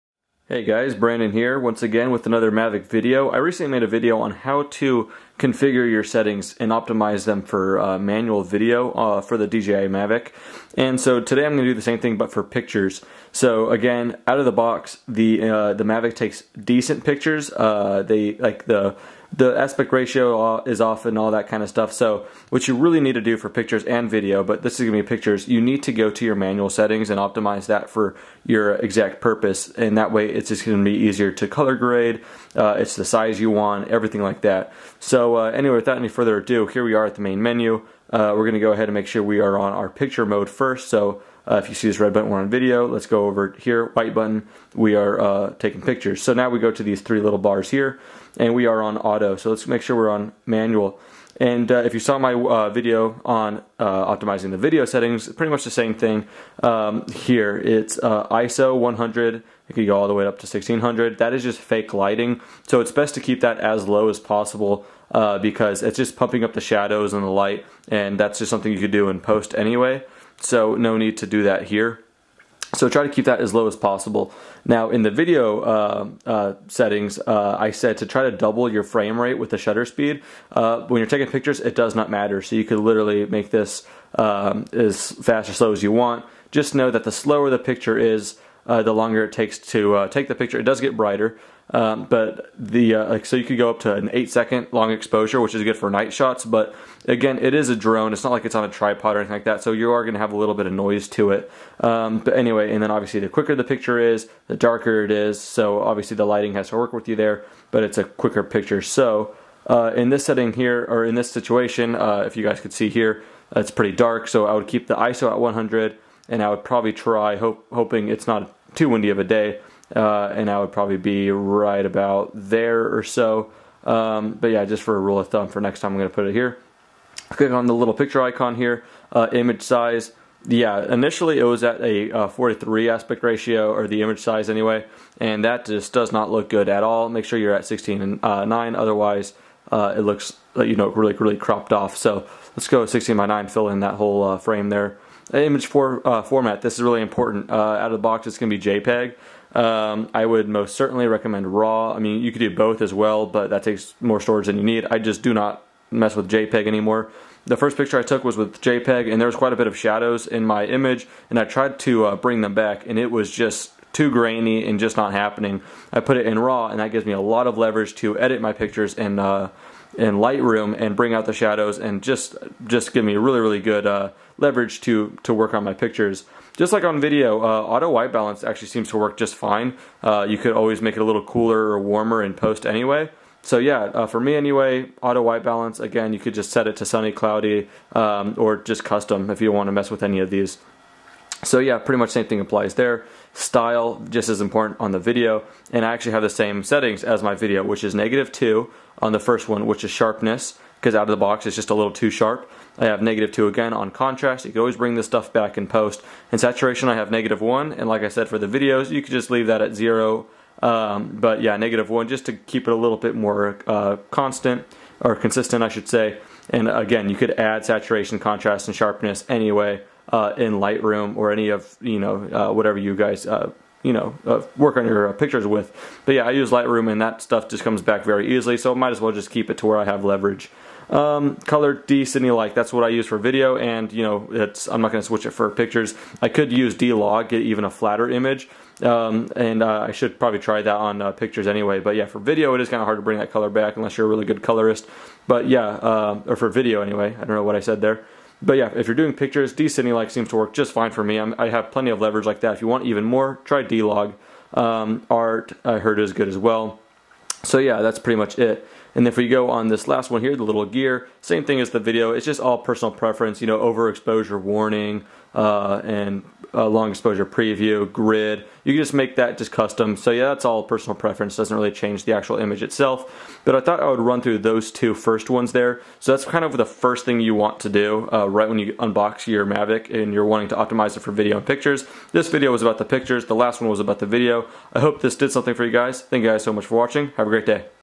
How To Set Up The DJI Mavic Pro For Pictures
In this video, I walk through my personal picture settings for my DJI Mavic Pro. I recommend you fly with your personal manual settings to get the best results.